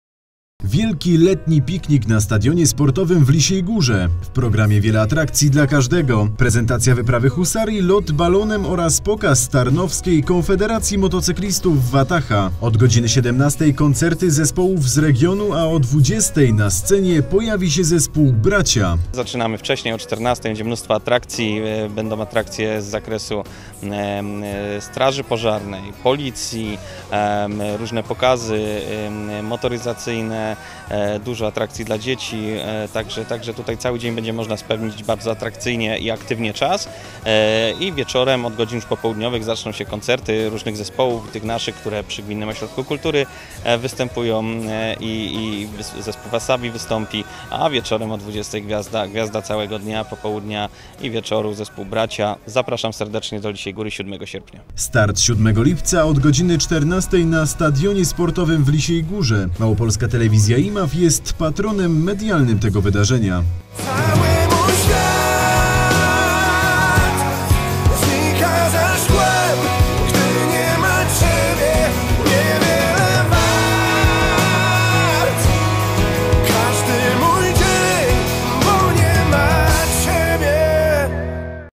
Bracia na Wielkim Letnim Pikniku w Lisiej Górze - Magazyn Miejski 03/08/2016 - imav.tv
Wielki Letni Piknik na Stadionie sportowym w Lisiej Górze. W programie wiele atrakcji dla każdego. Prezentacja wyprawy husarii, lot balonem oraz pokaz Tarnowskiej Konfederacji Motocyklistów Wataha. Od godziny 17:00 koncerty zespołów z regionu. A o 20:00 na scenie pojawi się zespół BRACIA.